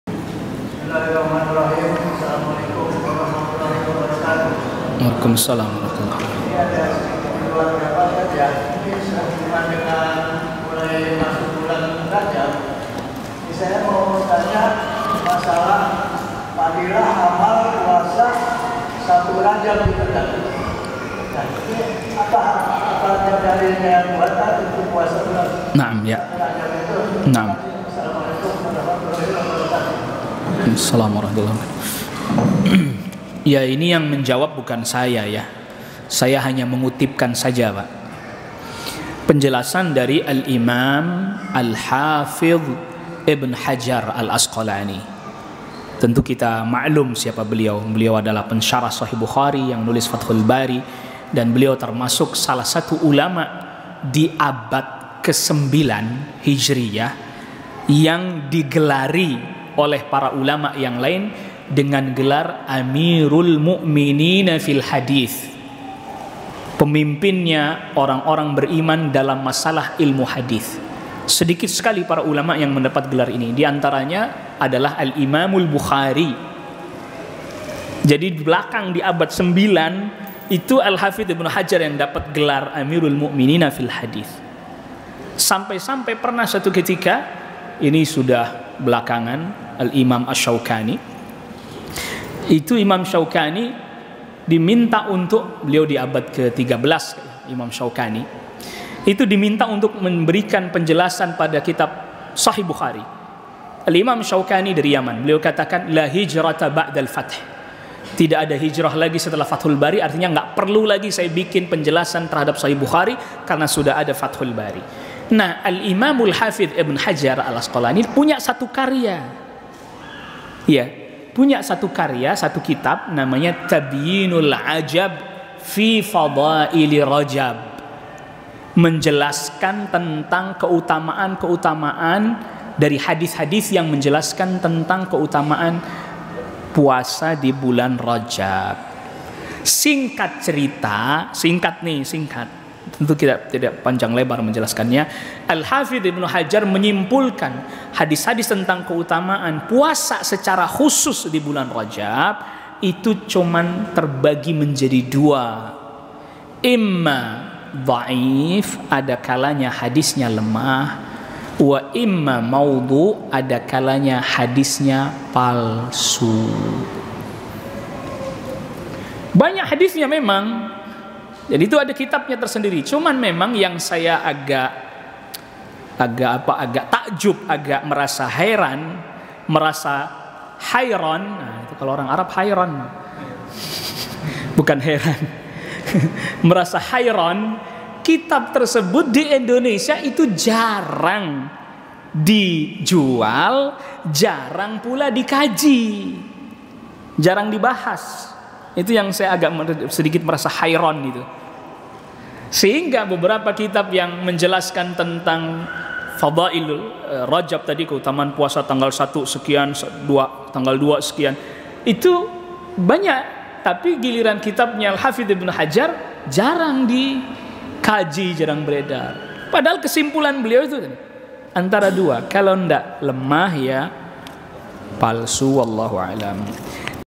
Tanya Jawab: Adakah Hadits Tentang Keutamaan Puasa Khusus di Bulan Rajab? - Ustadz Zainudin Khuzairi
Full Kajian:
"Larangan Merugikan Orang Lain (Hadits ke 32)"- Ustadz Zainudin Khuzairi